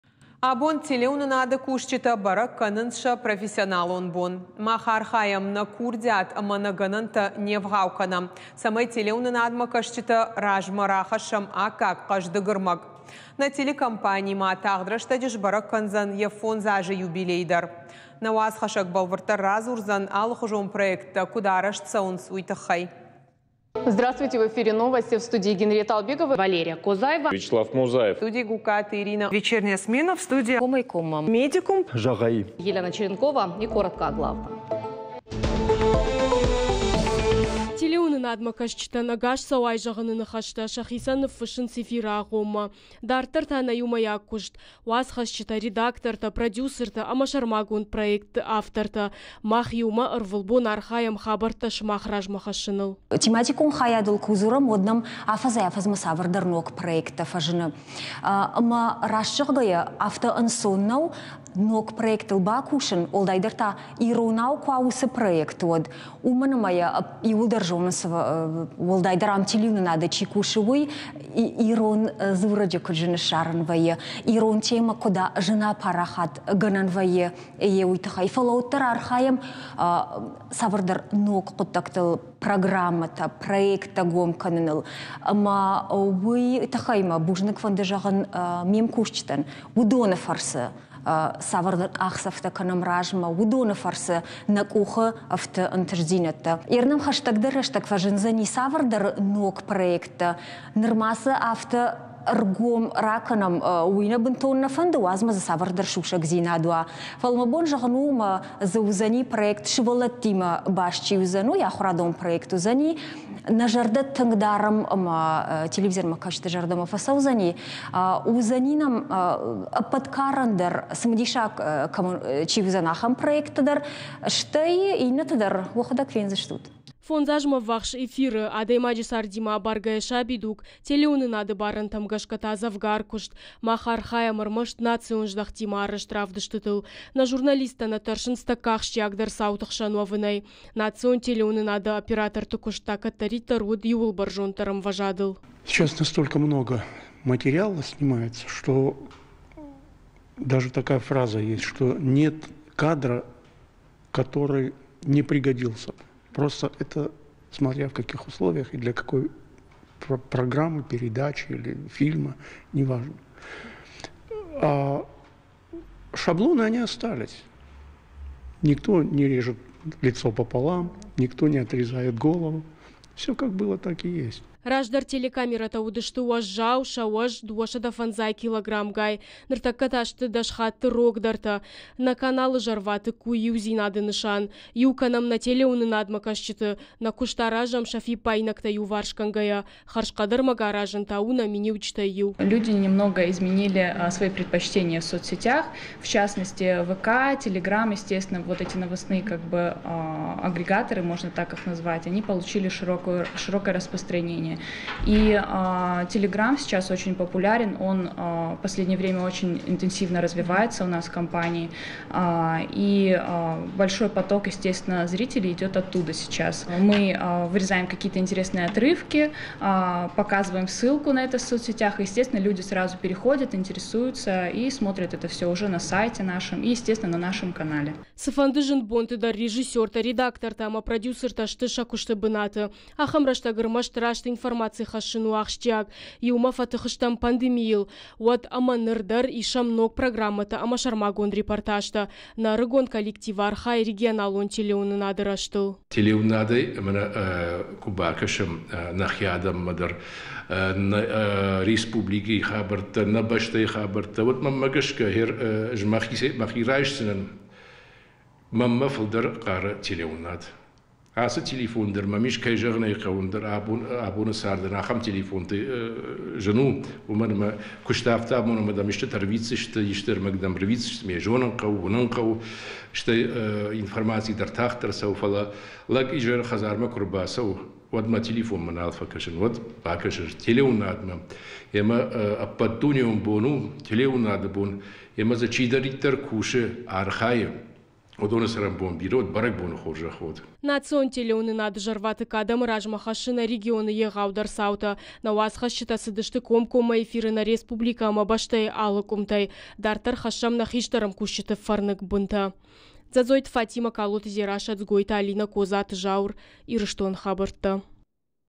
Бæрæгбон нысангонд æрцыд 1996 азы
Абон телеуынынады кусджытæ бæрæг кæнынц сæ профессионалон бон. Мах архайæм, нæ курдиат æмæ гæнæнтæ не 'вгъау кæнæм, цæмæй телеуынынадмæ кæсджыты размæ рахæхссæм аккаг, хъæздыг æрмæг. Нæ тедекомпани ма тагъд рæстæджы сбæрæг кæндзæн йæ 5 азы юбилей дæр. Нæ уацхæссæг бæлвырддæр радзурдзæн алыхуызон проекттæ куыд арæзт цæуынц, уый тыххæй.

Телеуынынадмæ кæсджытæн «æгас цу» зæгъайы ныхæстæ, сæхицæн ныффыссынц эфиры агъоммæ. Дарддæр та нæ иумæйаг куыст. Уацхæсджытæ, редактортæ, продюссертæ æмæ сæрмагонд проектты автортæ. Мах иумæ, æрвылбон архайæм хабæрттæ сымах размæ хæссыныл.

5 азмæ 'ввахс эфиры. Адæймаджы цардимæ абаргæйæ — сабидуг, телуынынады барæнтæм гæсгæ та, дзæвгар куыст. Мах архайæм æрмæст национ здæхтимӕ арӕзт равдыстытыл. Нæ журналисттæ нæ тæрсынц тæккæ ахсджиагдæр цауты 'хсæн уæвынæй. Национ телеуынынады операторты куыст та кæддæриддæр уыд иуыл бæрзонддæр æмвæзадыл.

Аивта кæнд техникæ нӕ. Раздæр уыдысты уæззау телекамерæтæ, сæ уæз 250 килограмгай. Ныртæккæ та сты дæс хатты рогдæртæ. Нæ каналы "Зæрватыкк" у иудзинады нысан. Иу кæнæм нæ телеуынанадмæ кæсджыты. Нæ куыст аразæм, сæ фиппаинæгтæ иуварсгæнгæйæ. Хæрзхъæд æрмæг аразын та у нæ миниуджытæй иу.

Ацы зын бонты режиссертæ, редактортæ æмæ продюссертæ сты сæ куысты бынаты. Ахæм рæстæг æрмæст раст информаци хæссын у ахсджиаг. Иумæ «фӕтых стӕм» пандемийыл. Уæд æмæ ныр дæр исæм ног программæтæ æмæ сæрмагонд репортажтæ. Нæ æрыгон коллектив архайы регионалон телеуынынады рæзтыл.

Ирыстон ТВ-йы зæрватыкк адæмы размæ хæссы нæ регионы егъаудæр цаутæ. Нæ корреспонденттæ цыдысты комкоммæ эфиры нæ республикæ æмæ бæстæйы алы къуымтæй. Дарддæр хæссæм нæ хистæр æмкусджыты фæрныг бынтæ.